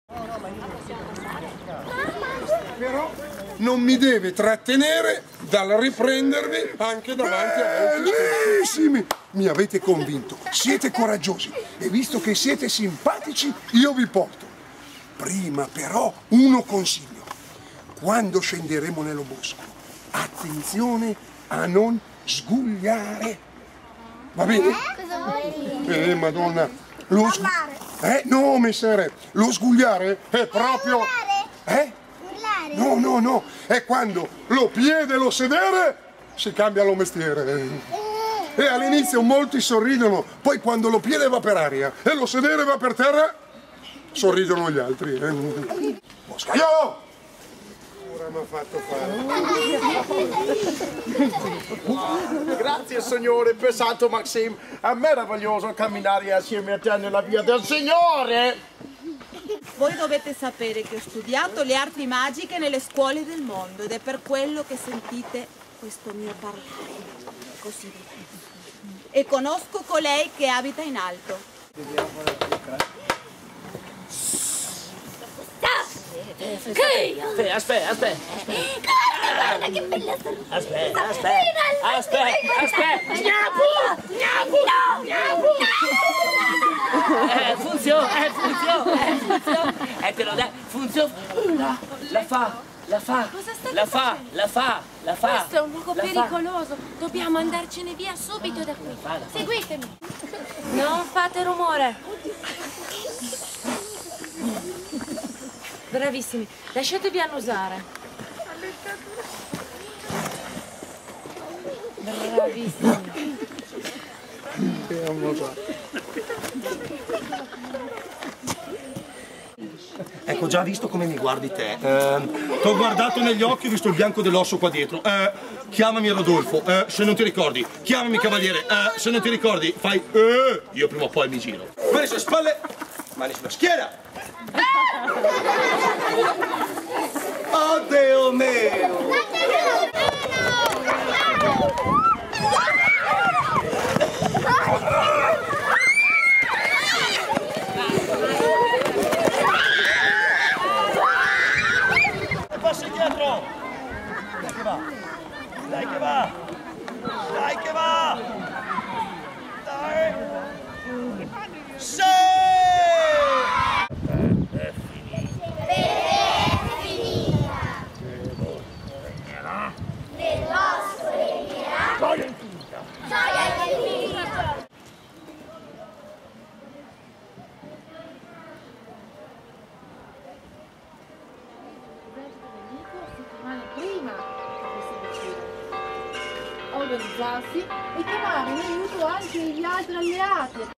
Al Castello di Gropparello nel Parco delle Fiabe
Una giornata di narrazione e animazione al Castello di Gropparello, sui colli di Piacenza. Reportage